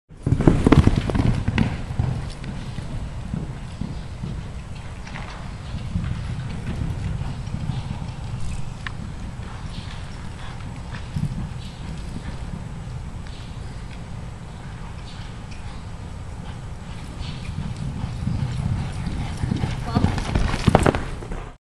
Muffin Aug 2011
2007 gelding by Perks Master. First exhibition away from home.